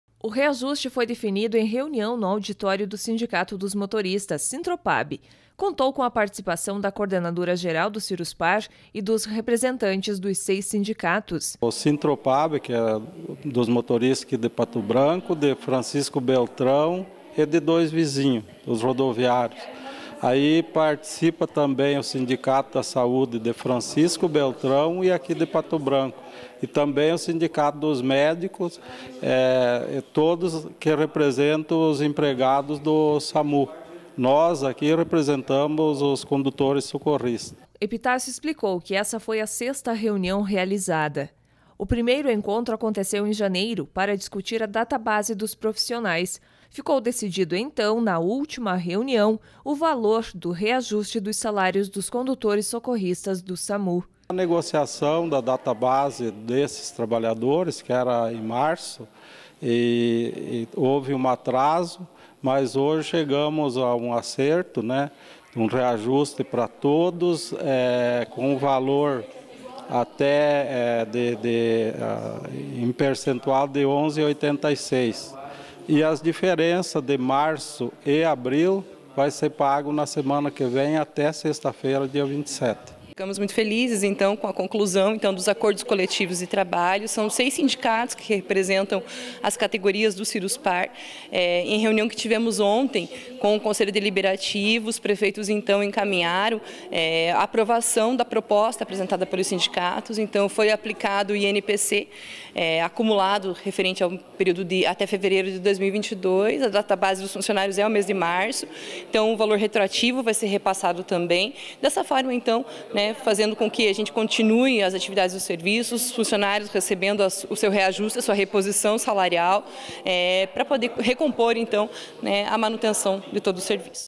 11,86% será concedido de reajuste aos condutores socorristas do SAMU
Segurança: Depois de várias reuniões ficou acertado o reajuste de 11,86% dos salários dos condutores socorristas do SAMU. E o retroativo de março e abril será pago até o dia 27 de maio.